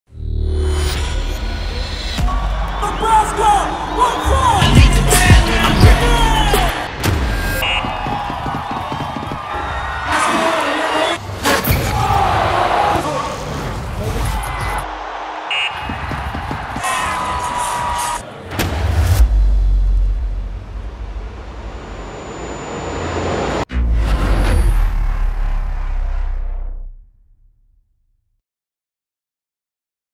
NSC 30 Recap 4k Inline Speed Skating
NSC 30 went off with a bang. Check out the recap of the amazing event.